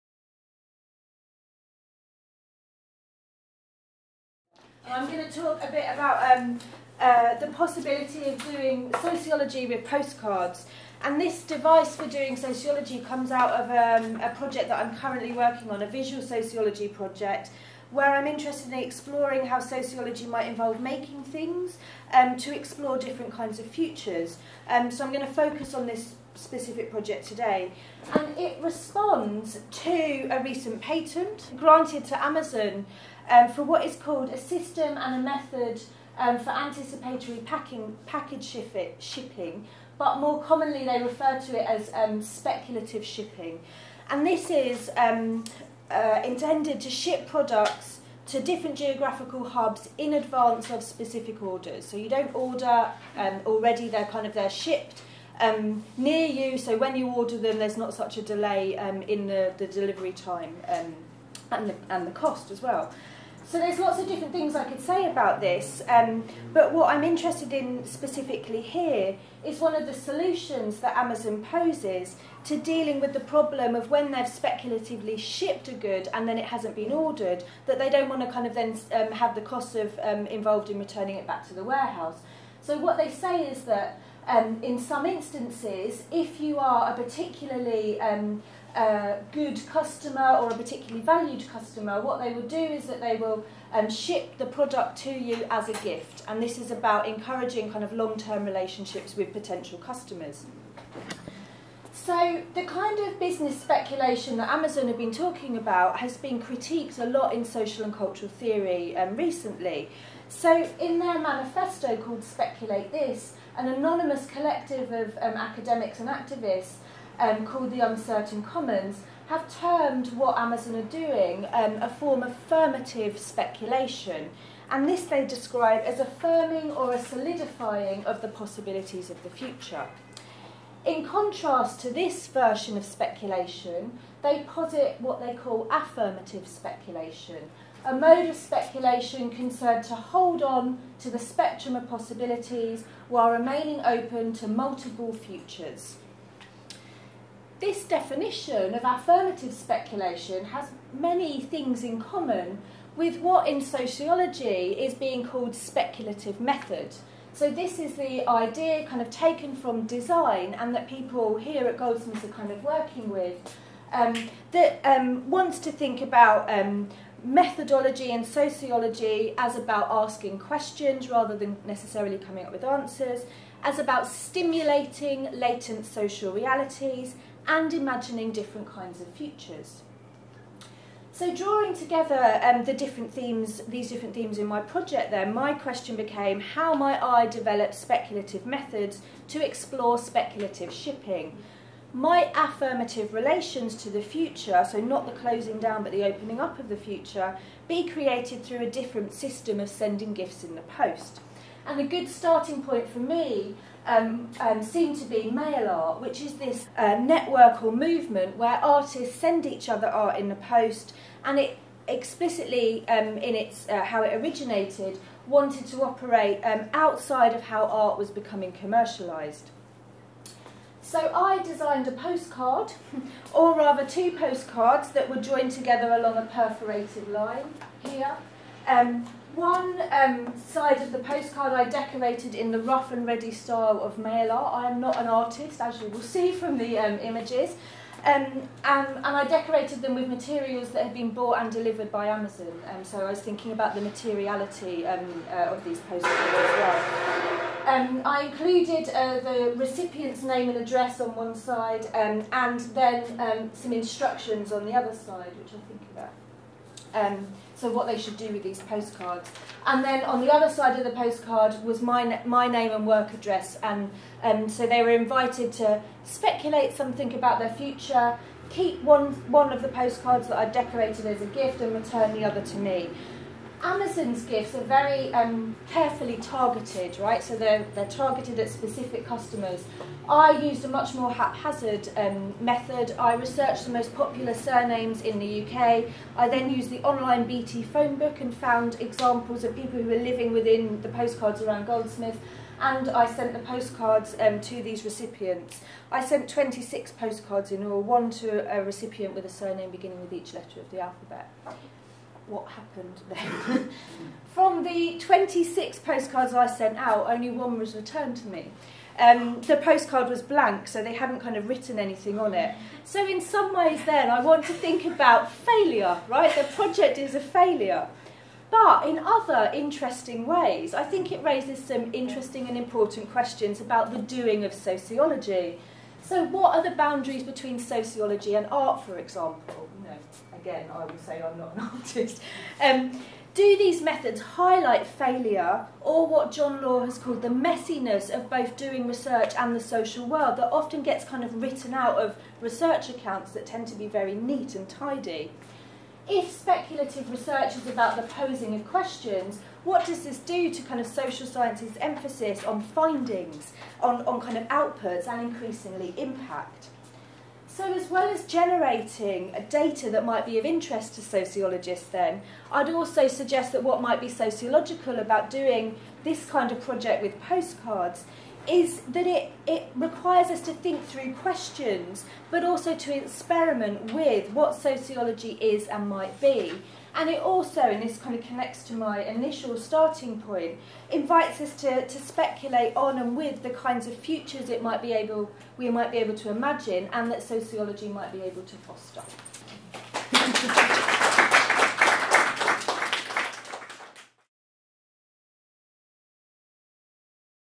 How to do Sociology with ... Postcards | Dr Beckie Coleman
At Goldsmiths, we practise Sociology with a variety of things, from buildings to music and social media. But how do we do this? Let us explain...